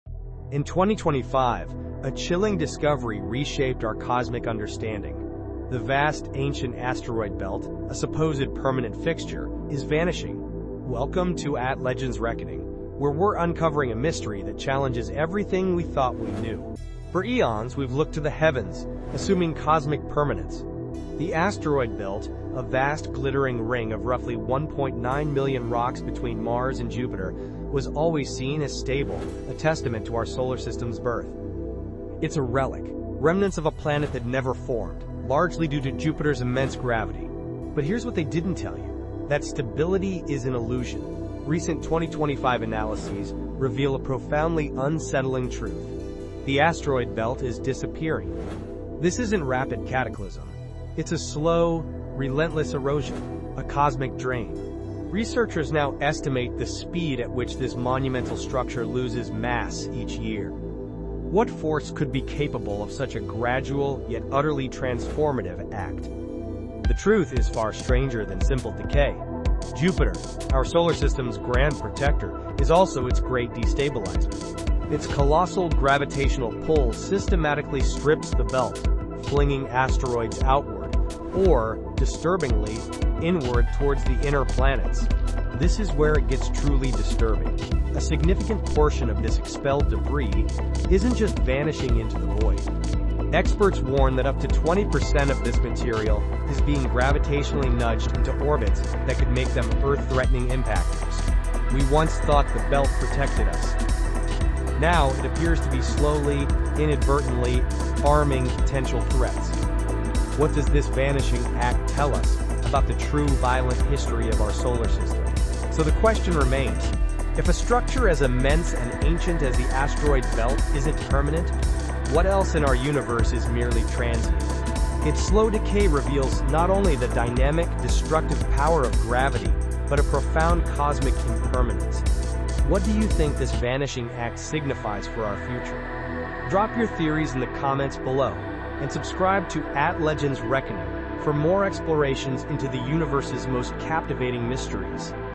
The Asteroid Belt Is Slowly Disappearing — Here’s Why
Our solar system’s asteroid belt is thinning over time. Collisions grind rocks to dust, Jupiter’s resonances and the Yarkovsky effect push fragments out, and material slowly escapes the belt. Here’s what drives the loss, the timescales involved, and what it means for missions and planetary defense.